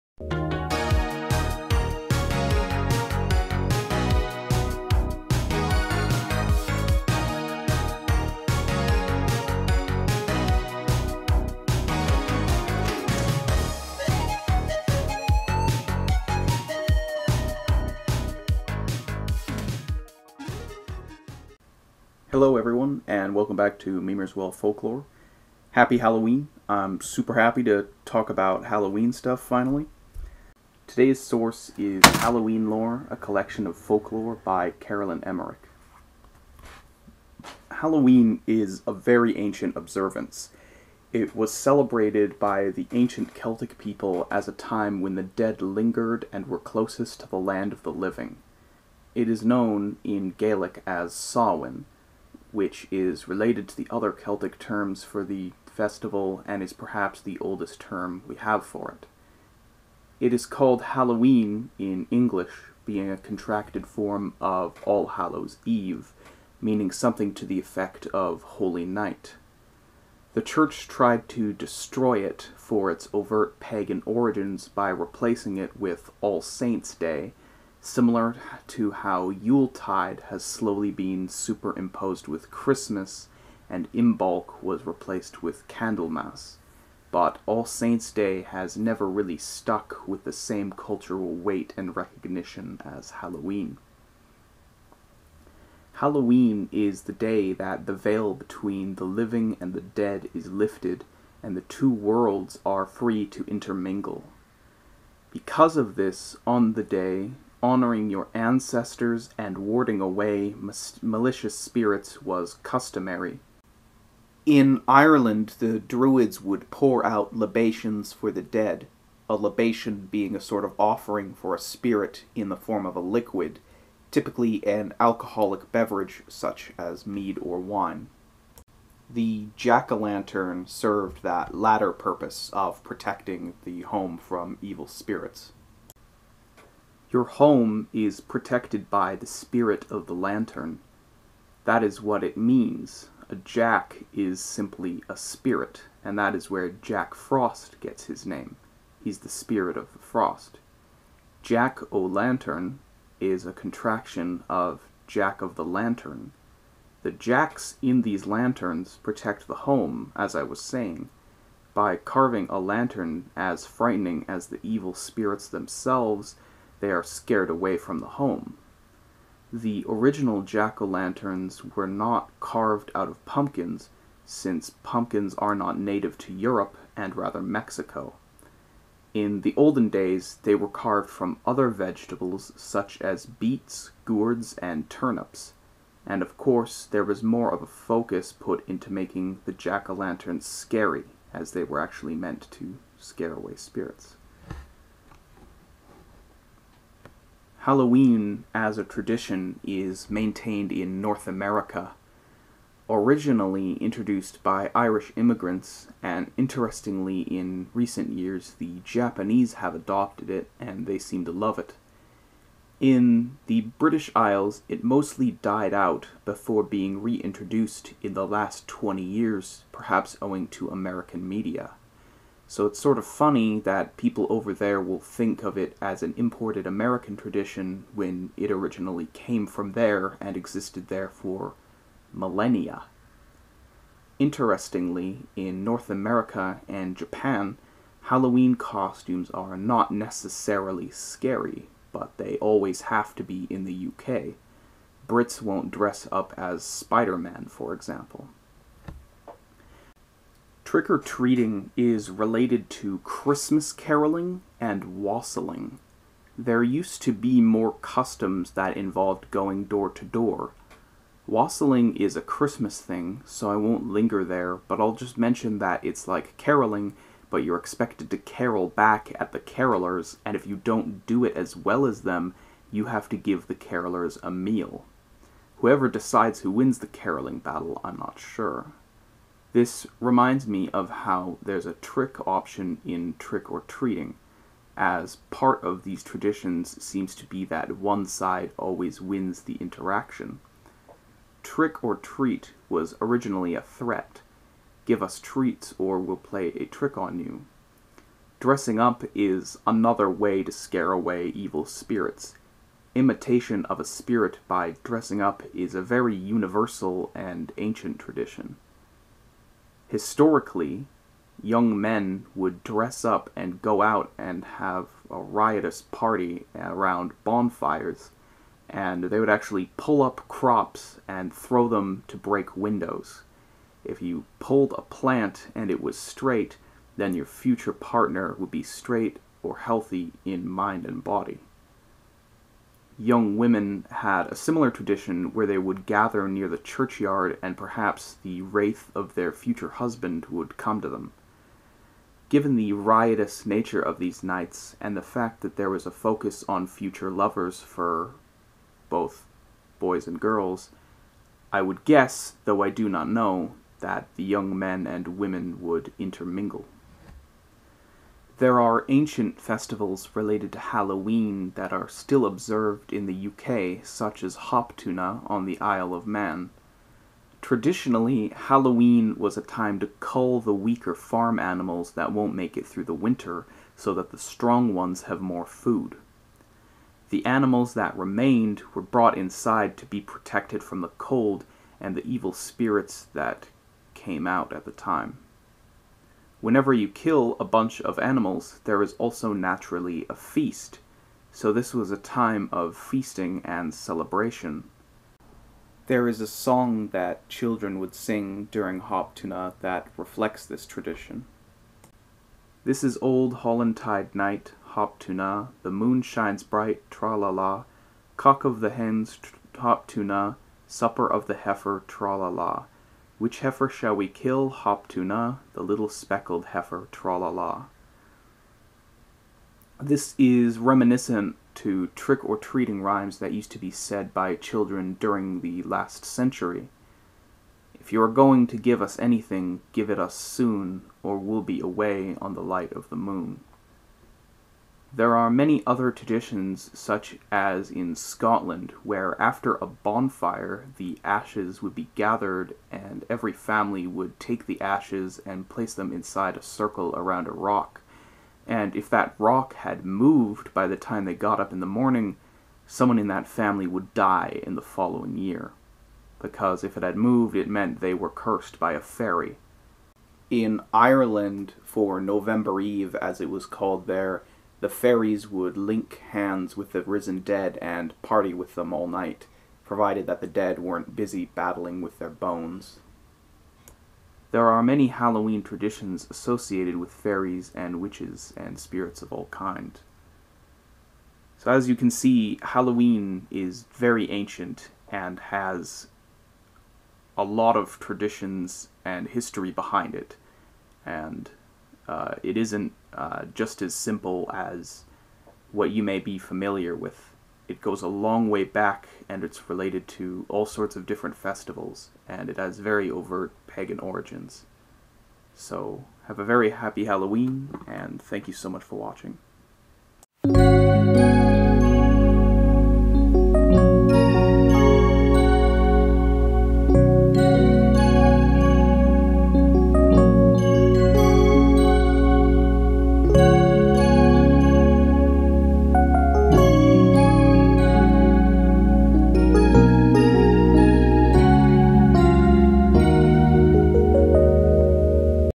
Why do we carve pumpkins? | The Origins of Halloween
Halloween is a very ancient and interesting holyday with a lot of background and customs associated with it from time to time and region to region. I discuss Halloween's origins and some of it's lesser known traditions and beliefs.

Today's sources include:

Halloween Lore a Collection of Folklore by Carolyn Emerick

A History of Irish Fairies by Carolyn White